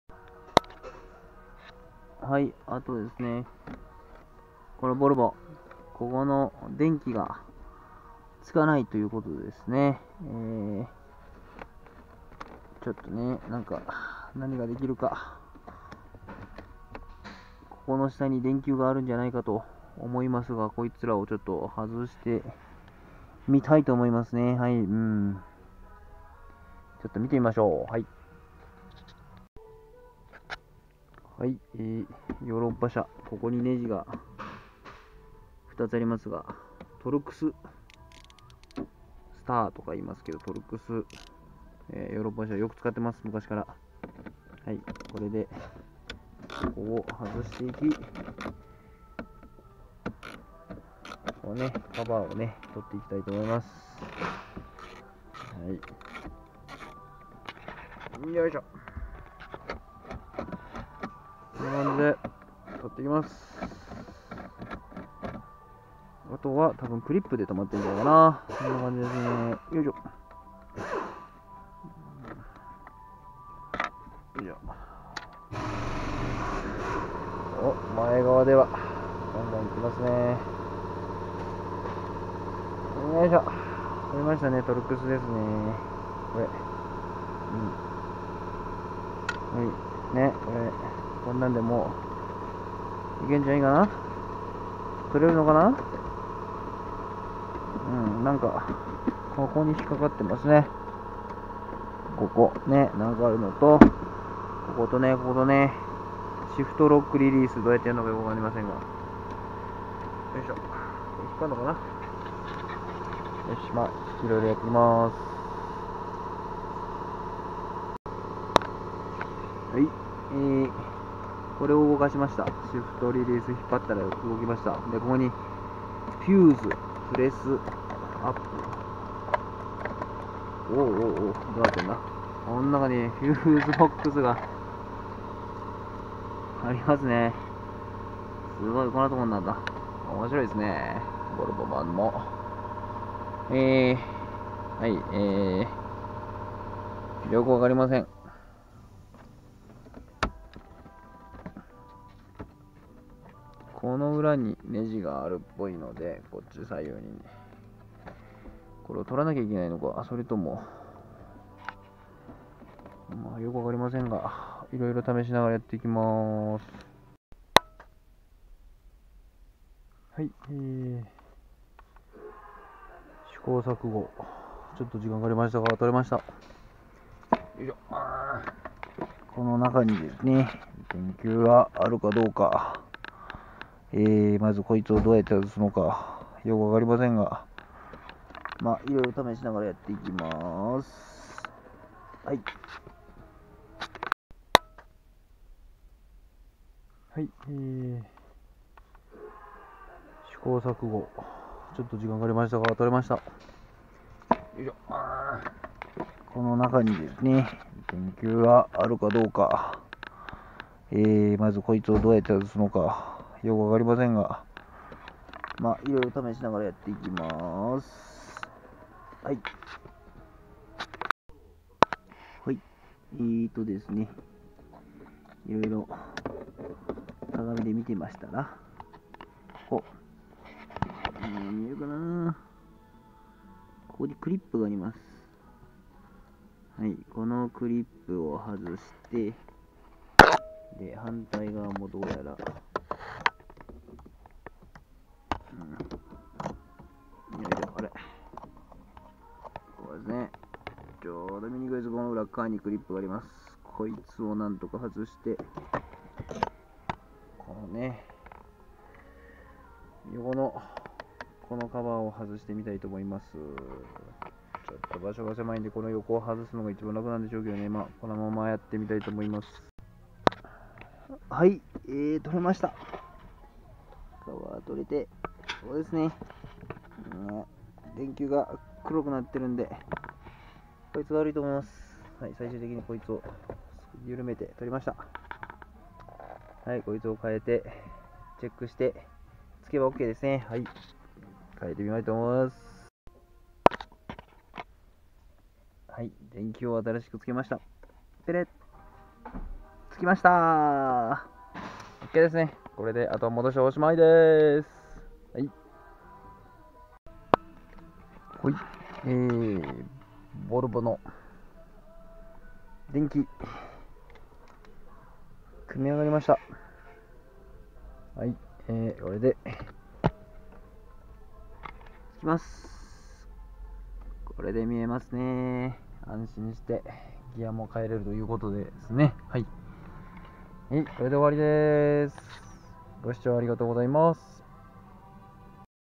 カナダで整備士 1991年 Volvo 740 オートマシフトインジケーター 電球交換。
1991年　ボルボ　タイミングベルトと一緒にオートマのシフトインジケーターの電球も交換しました。